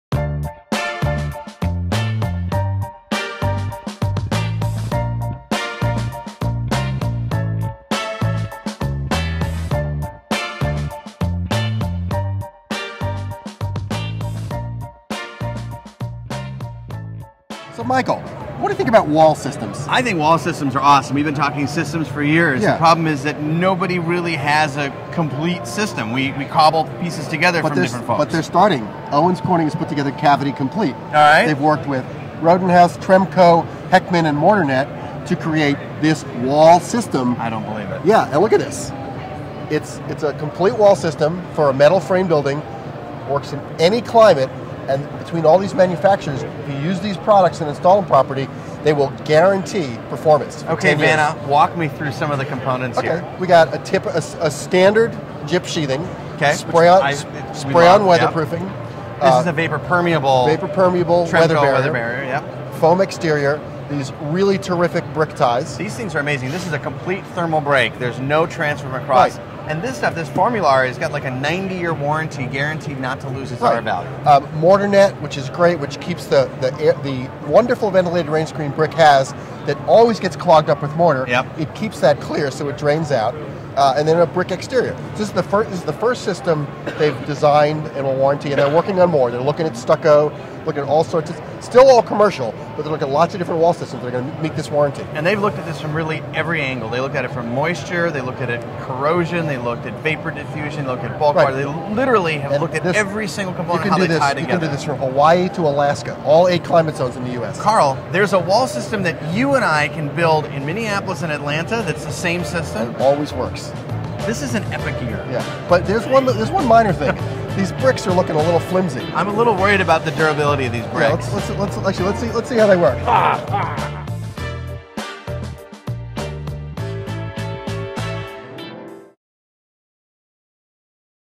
The Green Police Break Down a Complete Wall System
Michael Anschel and Carl Seville agree that wall systems are great. In this segment they look at the components of CavityComplete, a new commercial wall system from Owens Corning.

Michael Anschel, CEO of Verified Green, and Carl Seville, principal at SK Collaborative, are the Green Police. In this series they scour the exhibit floor at the 2014 Greenbuild International Conference and Expo on the hunt for products you can truly call "green." Watch the duo’s antics while hearing their assessments of noteworthy products.